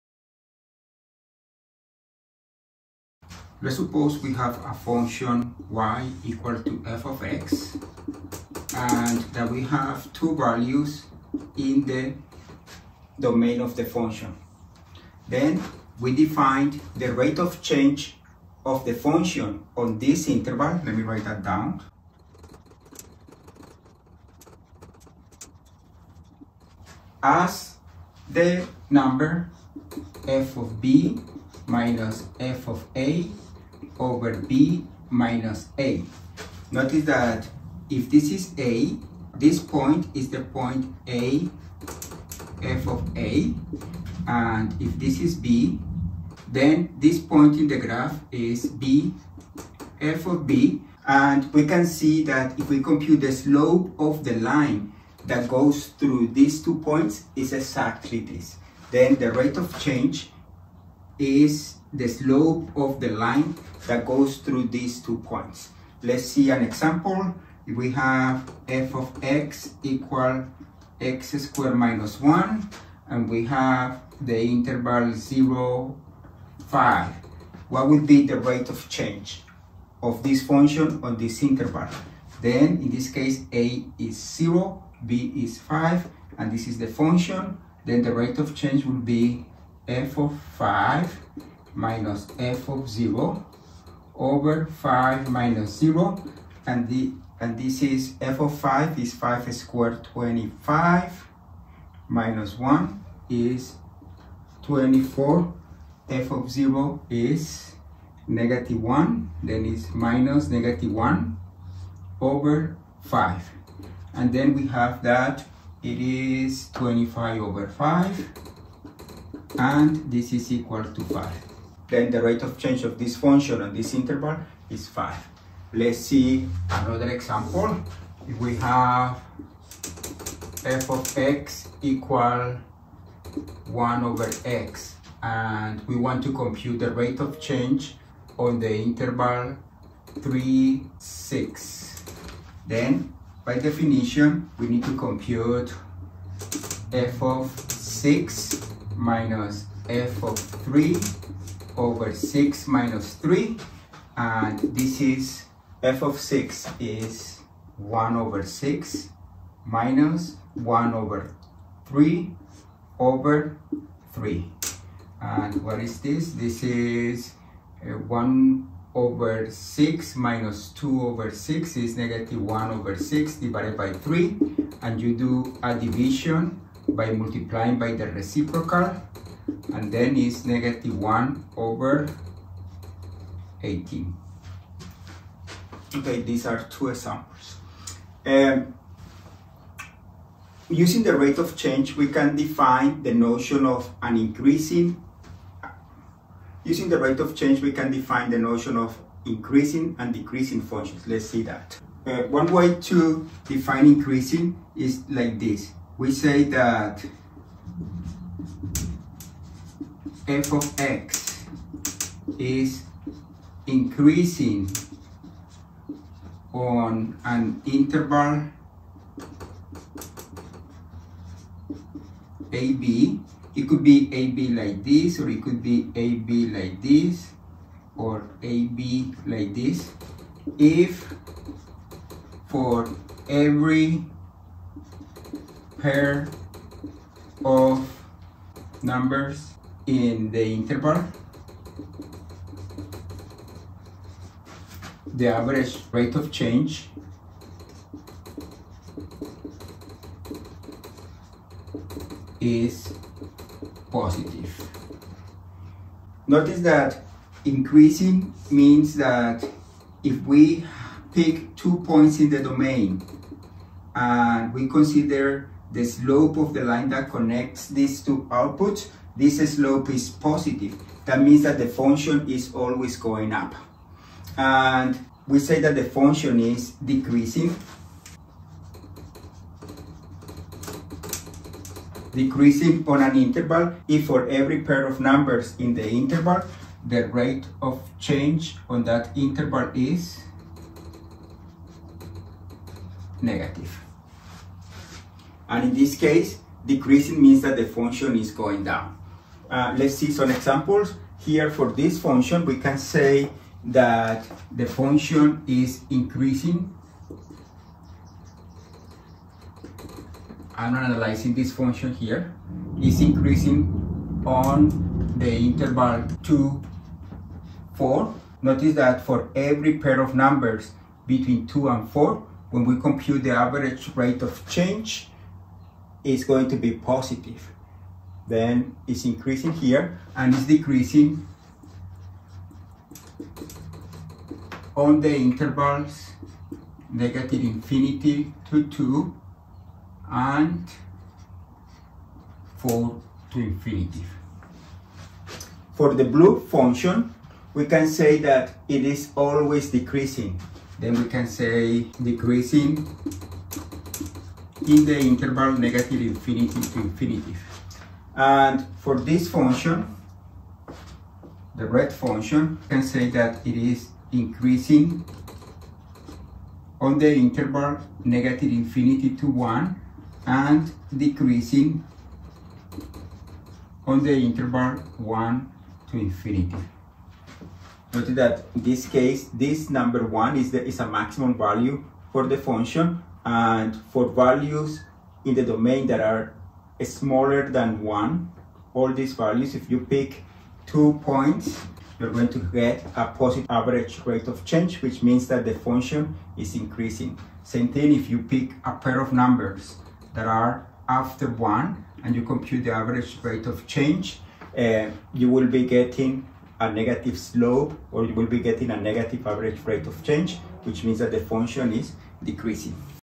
Average rate of change, increasing and decreasing functions
We do examples of the average rate of change and from the graph, we decide the intervals where a function is either increasing or decreasing.